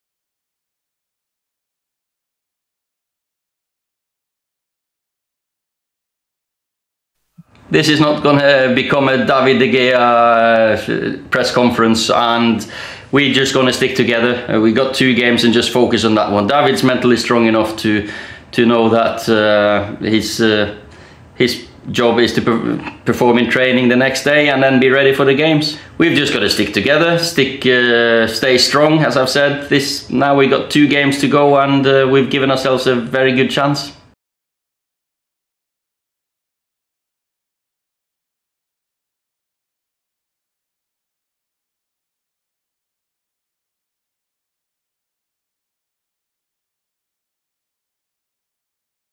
Solskjaer Backs 'Mentally Strong' David De Gea
Manchester United boss Ole Gunnar Solskjaer previews his side's Premier League clash with West Ham on Wednesday.

The Red Devils manager says that David de Gea is 'mentallty strong' after the goalkeeper's errors against Chelsea in Sunday's FA Cup semi-final defeat.